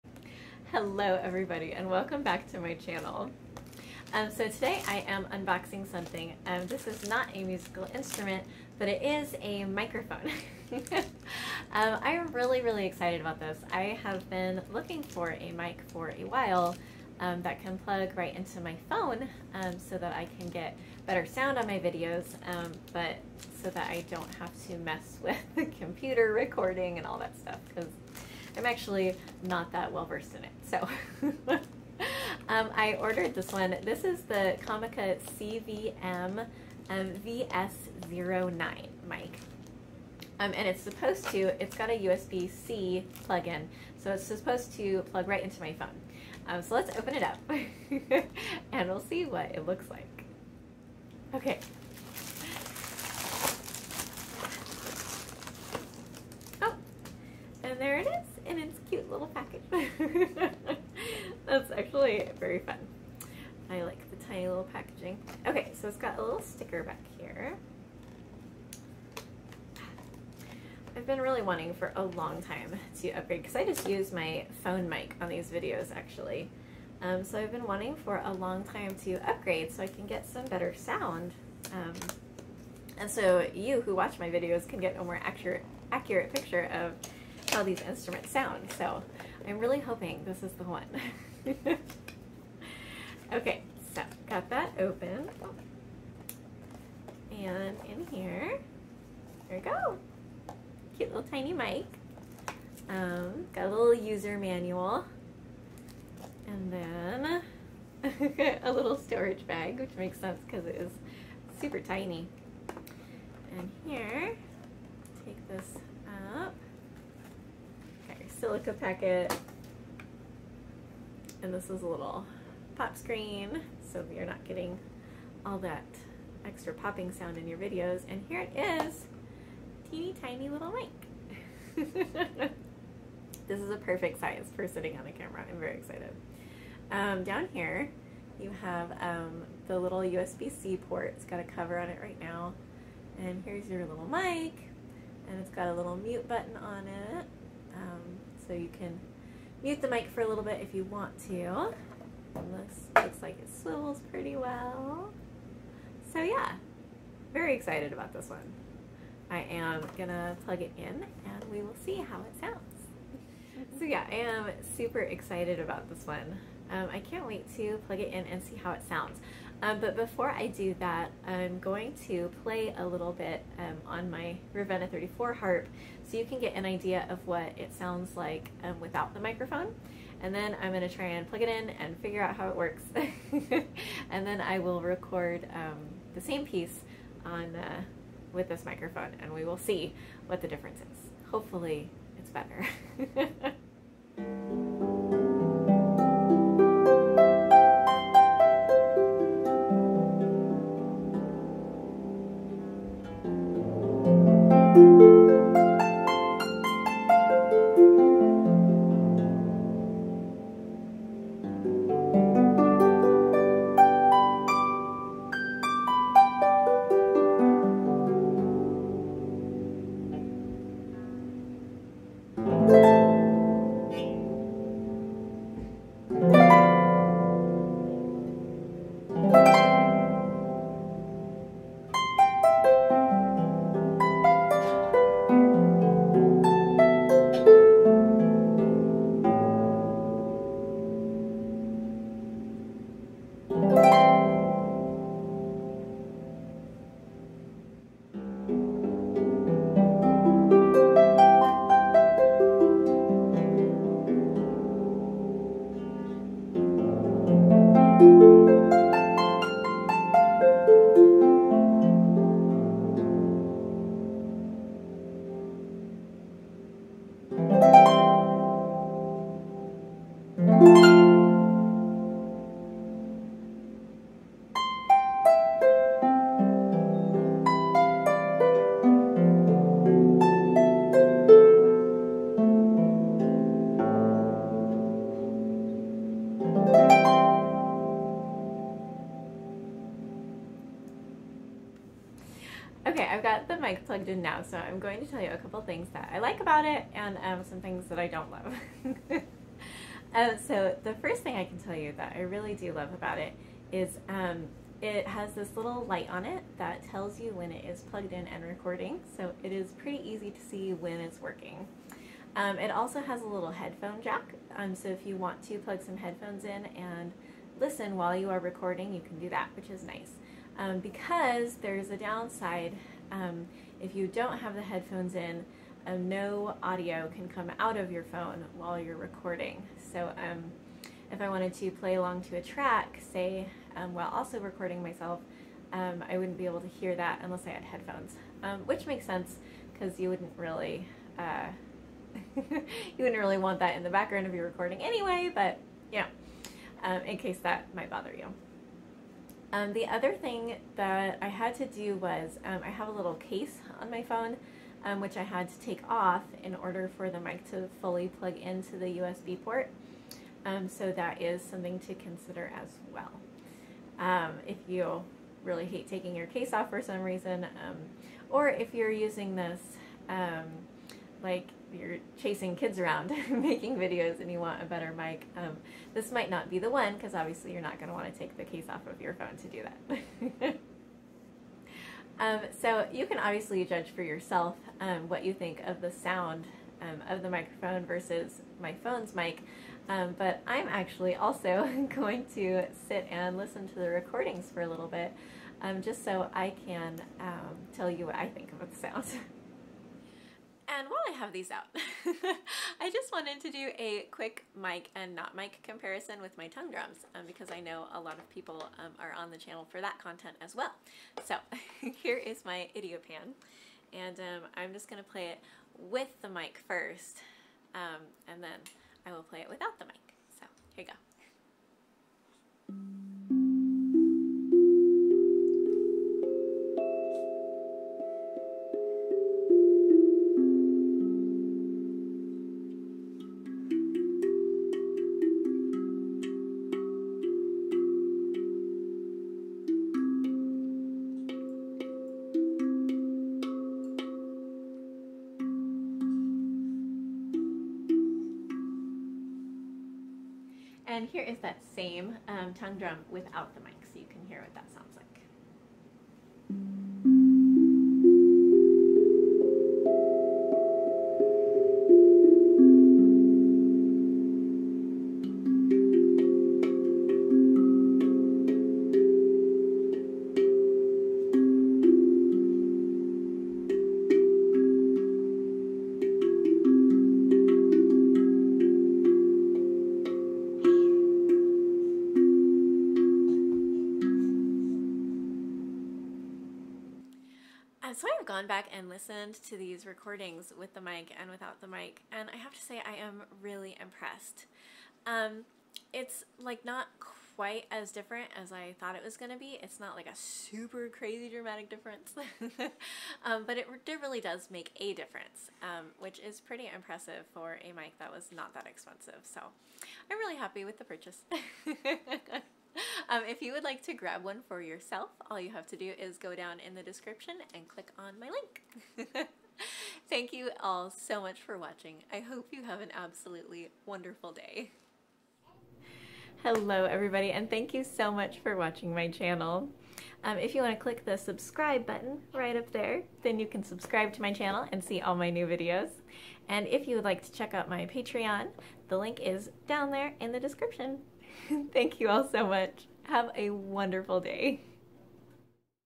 Comica CVM-VS09 Phone Microphone Unboxing and Review
Today I'm unboxing a super sleek USB-C Microphone for my phone!

I could not find the USB equivalent so let me know if you see it!